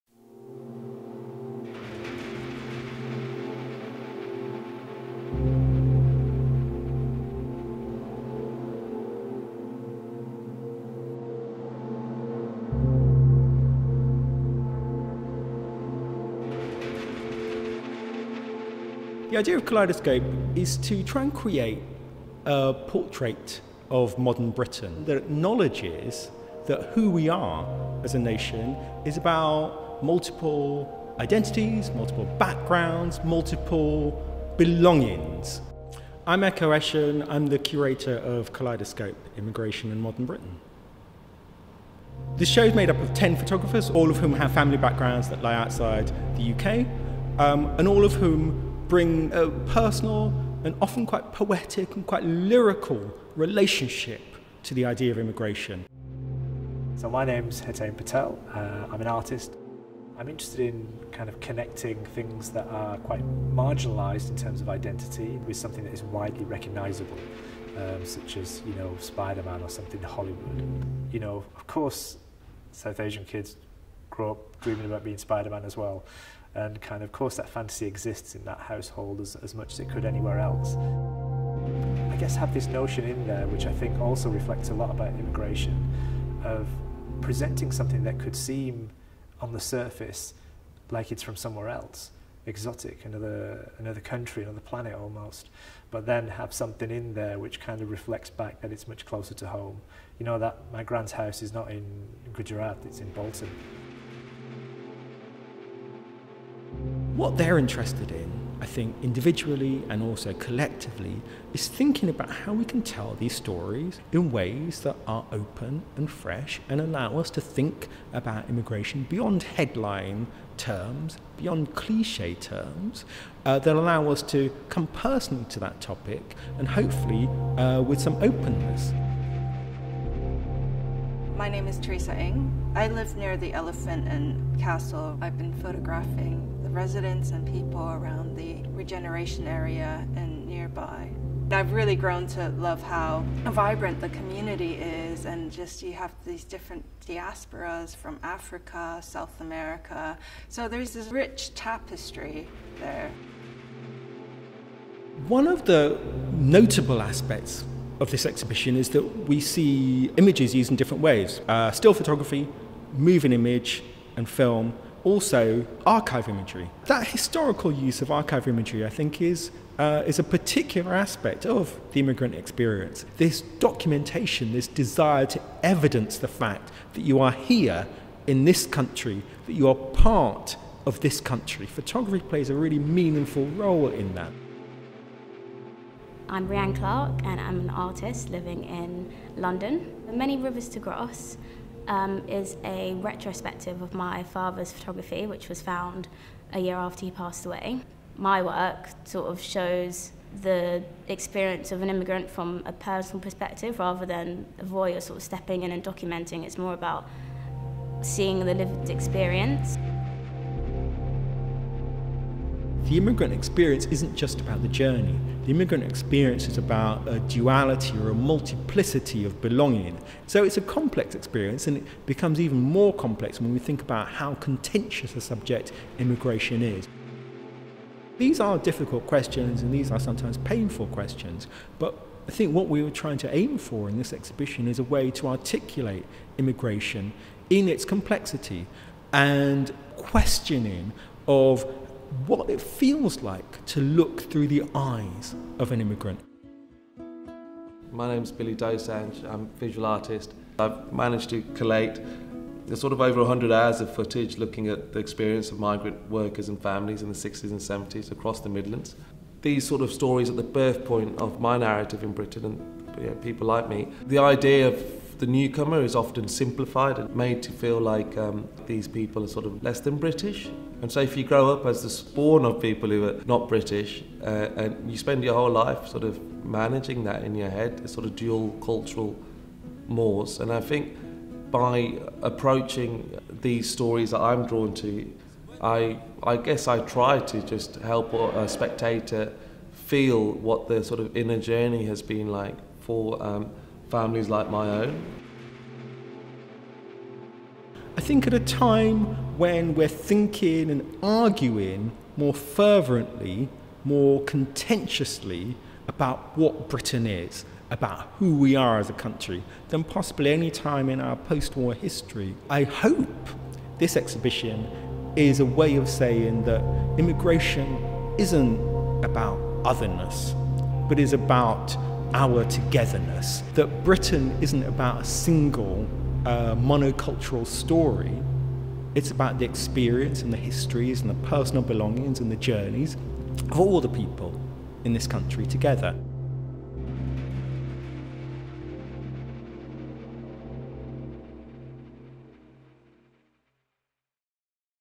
Kaleidoscope | Immigration and Modern Britain
A new photography exhibition exploring identity and immigration in modern Britain. 12 Jun – 08 Sep 2019 at Somerset House

Featuring stills and moving image, Kaleidoscope showcases the works of ten photographers born or based in Britain, many with family origins abroad including Hong Kong, India, Jamaica and Russia, and explores what it means and how it feels to live as an immigrant, or a descendent of immigrants, in Britain today. Co-curated by writer, Ekow Eshun and Creative Director, Darrell Vydelingum the exhibition forms a celebration of immigration in everyday life.

Reflecting the multiplicity of voices that together form modern Britain, the exhibition takes individual and often intensely personal experiences to encourage a wider appreciation of the nation’s multiculturalism. The significance of immigrant communities forms a key focus, particularly how they influence the country’s identity, challenged now more than ever.

FEATURED ARTISTS & WORKS
Informed by his own history as an illegal immigrant, Seba Kurtis presents Heartbeat, a series of portraits of migrants held at UK detention centres, inspired by the British police’s use of heartbeat detectors to locate people hiding amongst cargo.

Chris Steele-Perkins’ large-scale The New Londoners studies families from each of the 200 UN-recognised countries of the world now living in London; the stories of their origins, and the reasons why they chose to settle in the city.

In Dalston Anatomy, Lorenzo Vitturi captures the energy in the convergence of different cultures at Ridley Road Market in East London. Combining portrait photography with abstract sculpture comprised of objects collected from the market, Vitturi reflects on the cultural evolution of Ridley Road and its unique combination of international influences.

For The Quiet Town of Tipton, Mahtab Hussain collaborates with the local South Asian community to capture life in the Sandwell borough following a racist attack on the Kanzul Iman mosque in 2013, offering a voice to those othered by xenophobic attitudes.

Teresa Eng explores the theme of belonging by documenting the people and places in Elephant & Castle, her home borough in London, offering a first-hand perspective of how second and third generation families view their local community.

Kurt Tong’s photo series The Queen, The Chairman and I is a personal documentation of his family’s journey from Hong Kong to the UK over the last 100 years.

Rhianne Clarke’s Many Rivers to Cross forms a retrospective of her father’s photography of his Caribbean community in 1970s-80s London, discovered only following his death in 2014.

Hetain Patel brings a playful touch to immigration discourse in his film work, The Jump by gathering 17 British Indian family members in his grandmother’s home in Bolton, where he and his relatives have lived since 1967.

Photographer Liz Johnson Artur focuses on the capital in Real…Times, which premiered at the 10th Berlin Biennale 2018, weaving narratives from London’s African diaspora, from the Black Lives Matter activist rallies to sequences from Black female collective, Born N Bread.

Billy Dosanjh’s Year Zero: Black Country transports viewers to 1960s West Midlands. His video work compiles archive newsreel footage with personal testimonies from economic migrants in his hometown of Smethwick, forming a platform for a community underrepresented in mainstream culture.

Kaleidoscope: Immigration and Modern Britain is the next exhibition in The Charles Russell Speechlys Terrace Rooms Series, an ongoing partnership with the leading law firm to present a wide range of free exhibitions reflecting the broad interests of both organisations.